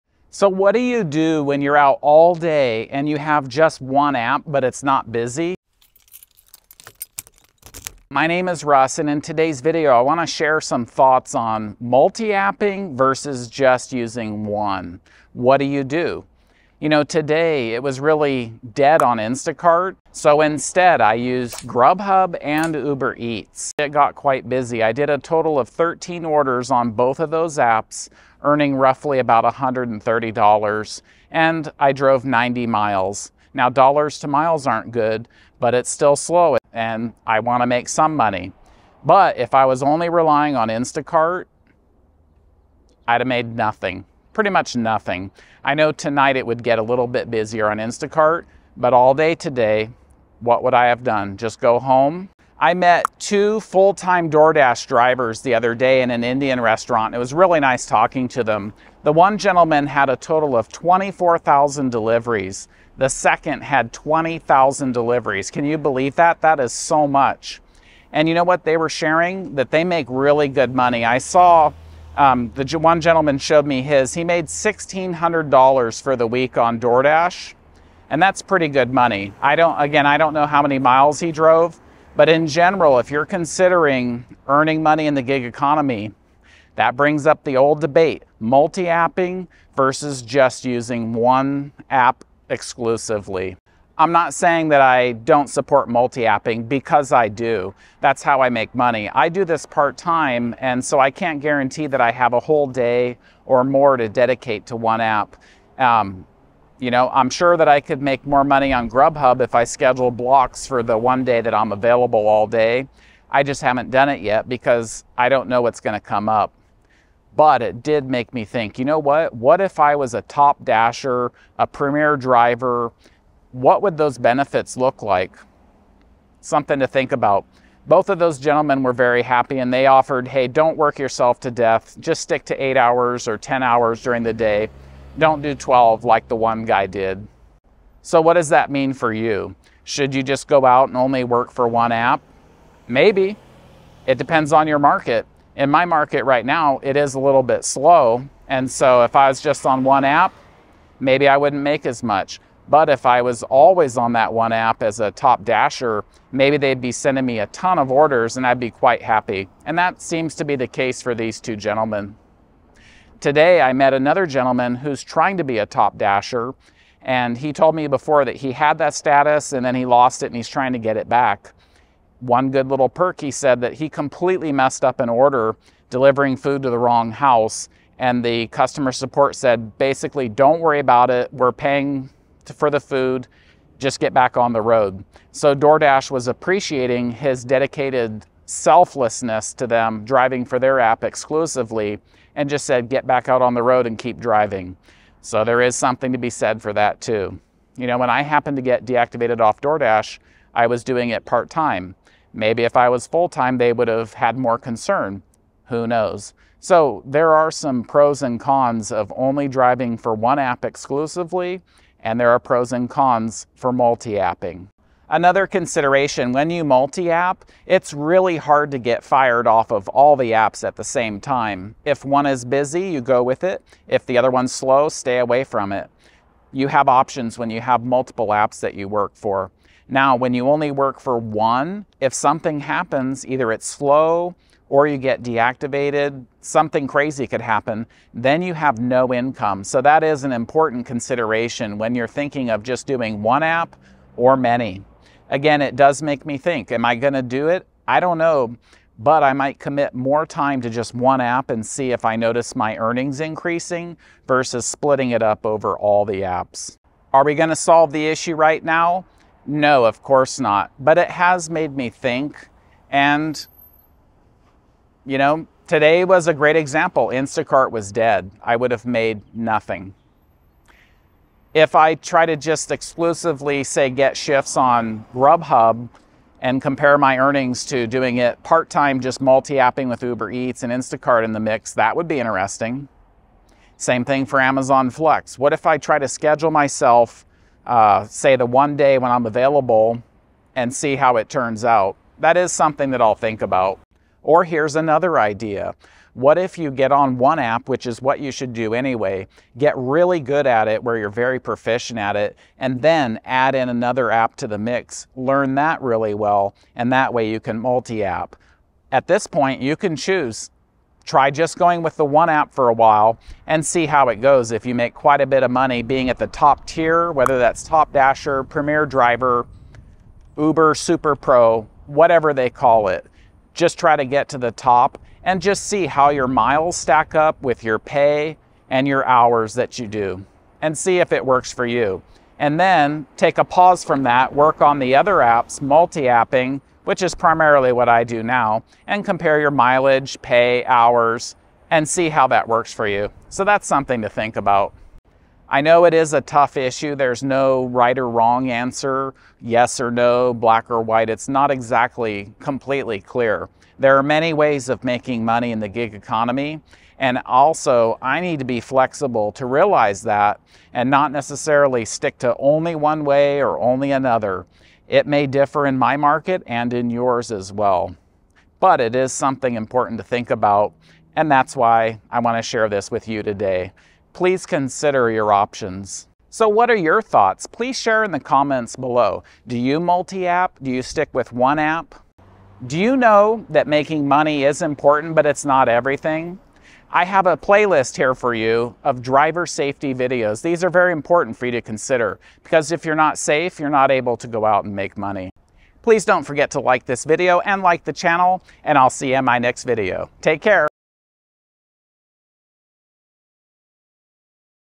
Multi App Vs. One App: The Pros And Cons
Gig economy workers, why isn’t there a clear answer?  Which is better, working for one app exclusively or muilt-apping (working for many apps)?  Have you considered splitting the difference from time to time?  In this video I will share some interesting information from three DoorDash drivers I met recently.  I will also contemplate my way ahead.  I think that the best way would be for me to get to a 100% acceptance rate to see if my experiment does result in higher paying orders.

I look forward to your comments and thank you in advance for your feedback!

Russ